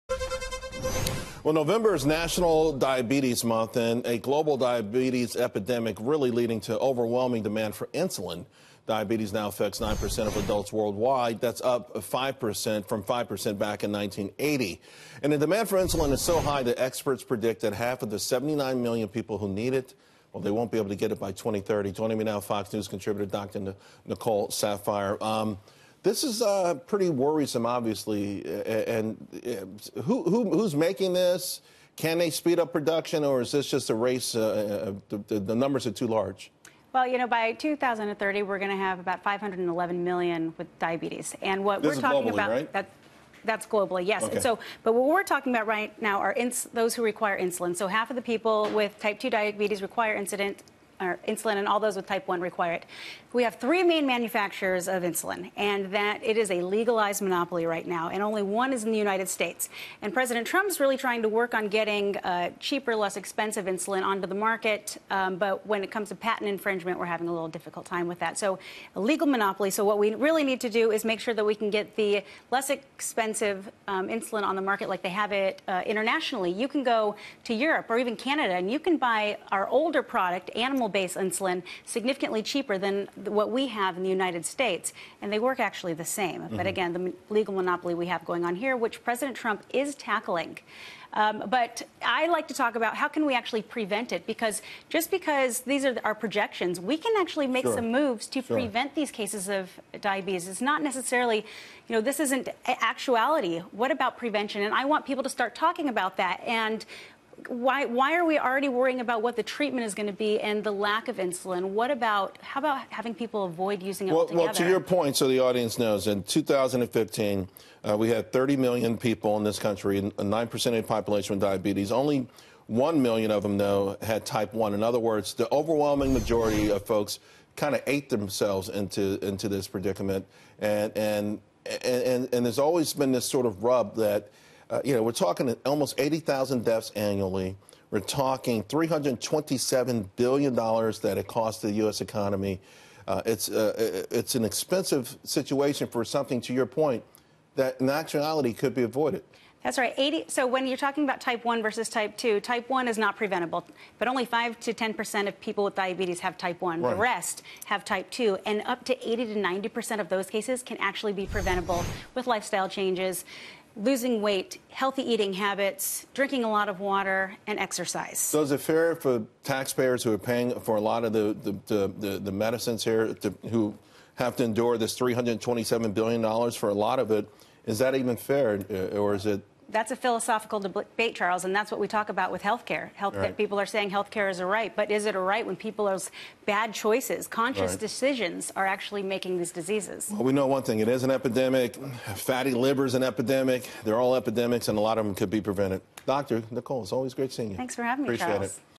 Diabetes epidemic fuels demand for insulin
Dr. Nicole Saphier on the global diabetes epidemic and the rising demand for insulin.